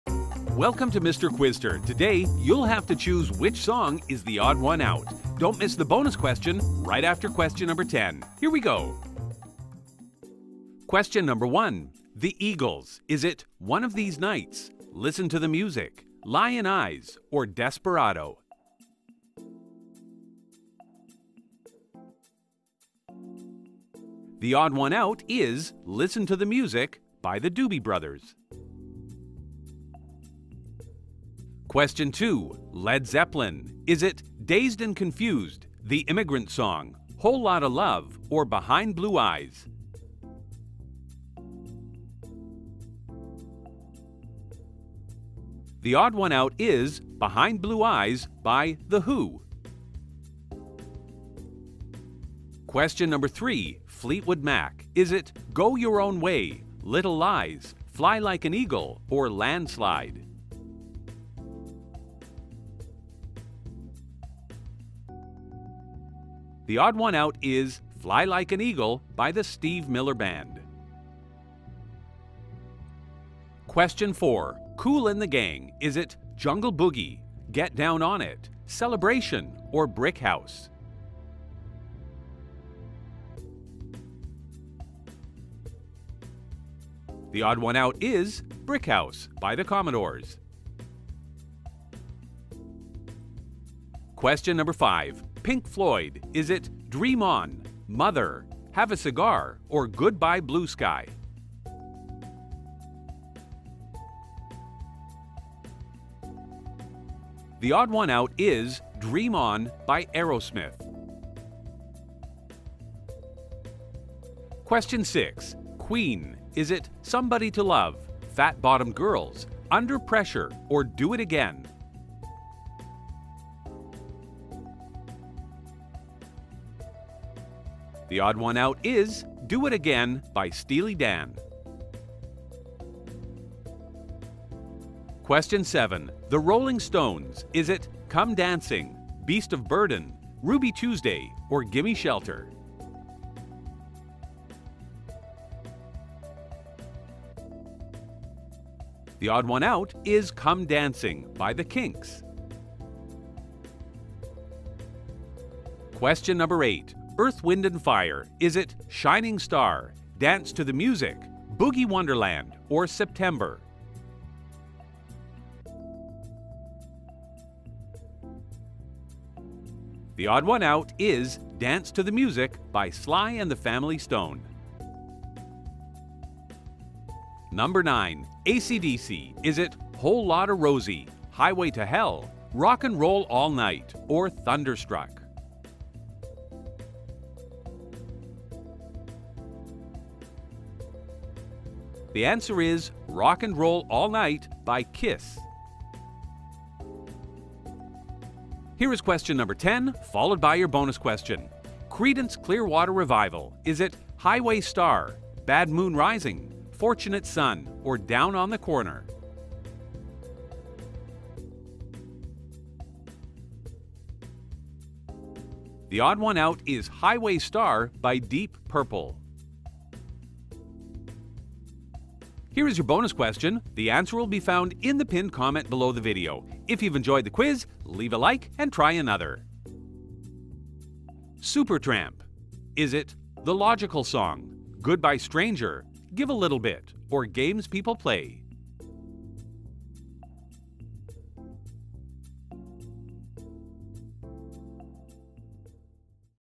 1970s MUSIC QUIZ (Can You Guess The Odd One Out?) 10 Multiple Choice Questions
1970s MUSIC Themed Trivia Questions and Answers with Multiple Choice.
No trick questions or silly riddles - just straight up old fashioned trivia.
If you like trivia, then give your brain a workout with Mister Quizster's Trivia Channel.

1970s Music Themed Quiz Questions
10 Questions, 4 Multiple Choice Options Plus A Bonus

🕘Timestamps:
0:00 Intro and Welcome
0:10 Q1 - Eagles
0:31 Q2 - Led Zeppelin
0:52 Q3 - Fleetwood Mac
1:13 Q4 - Kool & the Gang
1:33 Q5 - Pink Floyd
1:54 Q6 - Queen
2:14 Q7 - The Rolling Stones
2:35 Q8 - Earth, Wind & Fire
2:56 Q9 - AC/DC
3:18 Q10 - Creedence Clearwater Revival
3:42 Bonus (find the answer in the pinned comment)